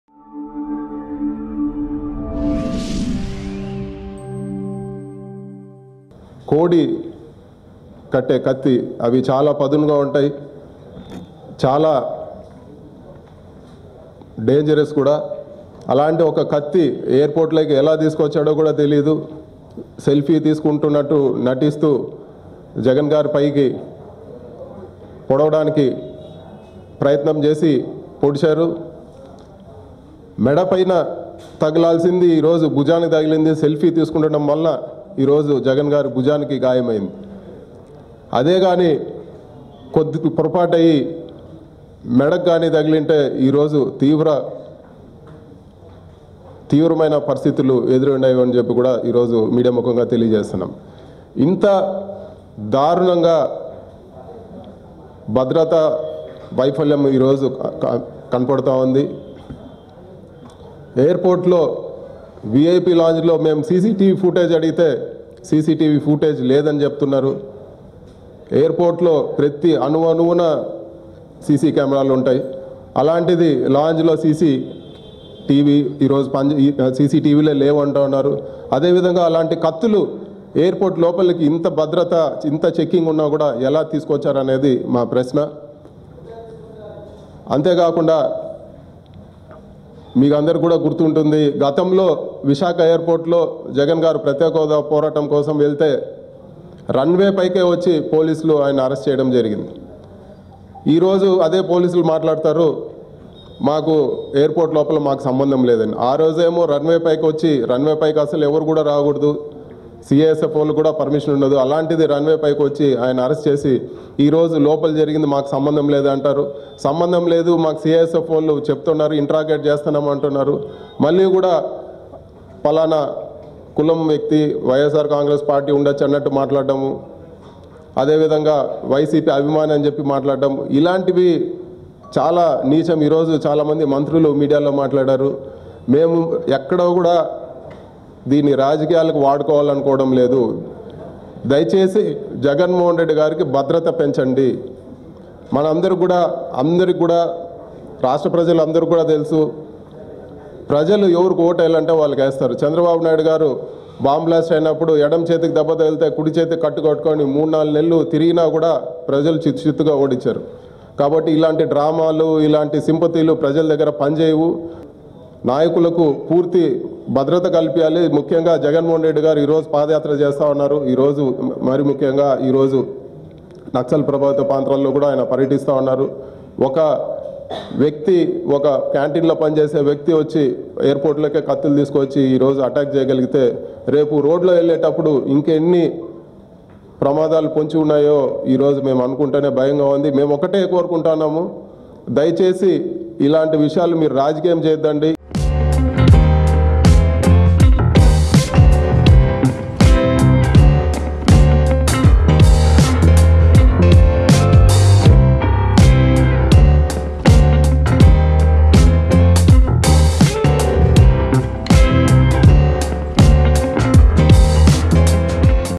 Ex MP Mithun Reddy Clarifies Over YS Jagan Assault with Knife at Vizag Airport | indiontvnews
Ex MP Mithun Reddy Clarifies Over YS Jagan Assault with Knife at Vizag Airport.